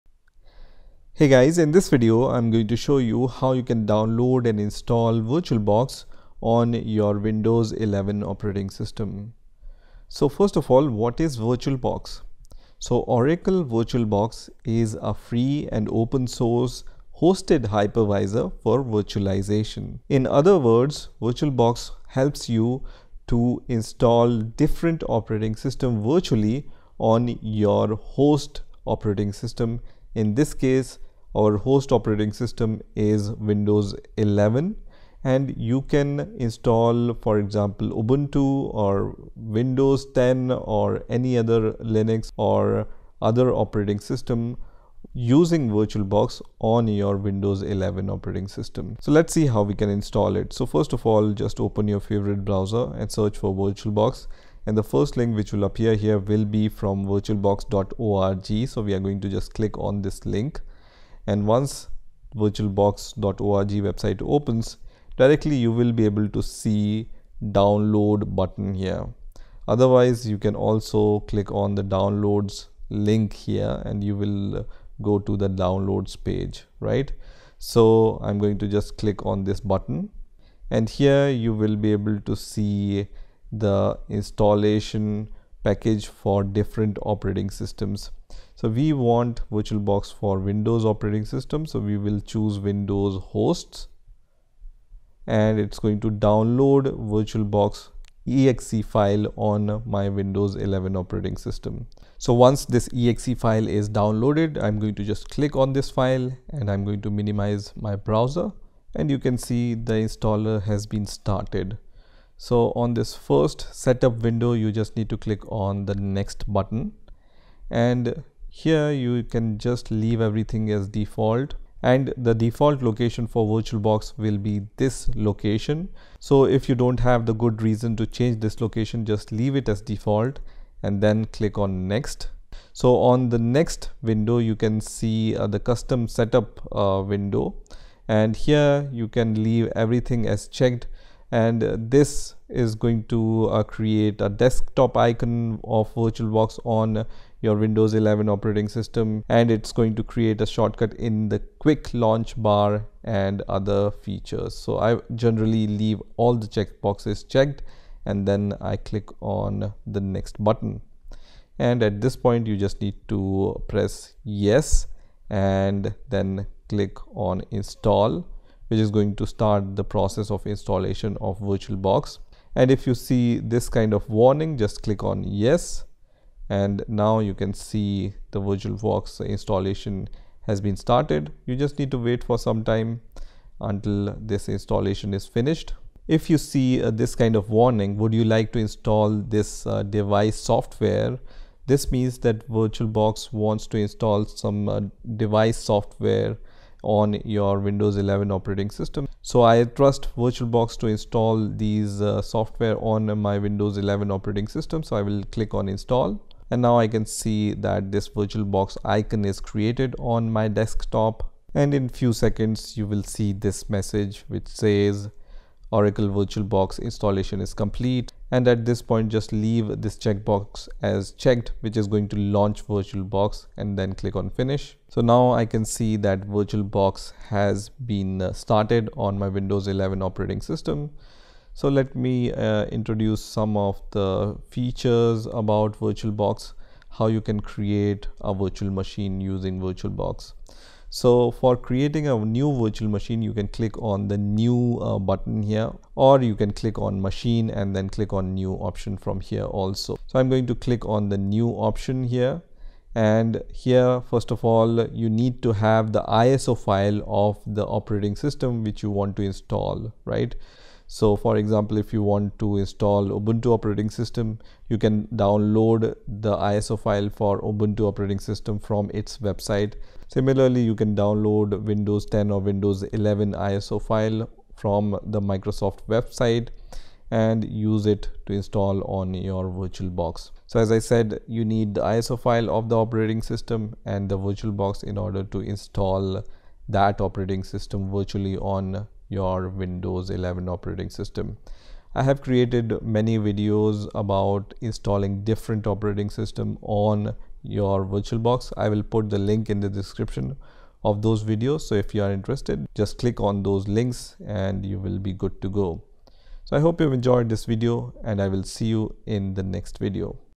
How to Install VirtualBox on Windows 11
In this video We are going to see How to Install VirtualBox on Windows 11. Installing VirtualBox on Windows 11  is simple and easy. first of all you need to Downloads – Oracle VM VirtualBox exe file from virtualbox.org website. Oracle VM VirtualBox (formerly Sun VirtualBox, Sun xVM VirtualBox and Innotek VirtualBox) is a free and open-source hypervisor for x86 computers currently being developed by Oracle Corporation. We are going to see How to install Oracle VirtualBox 6 in Windows 11.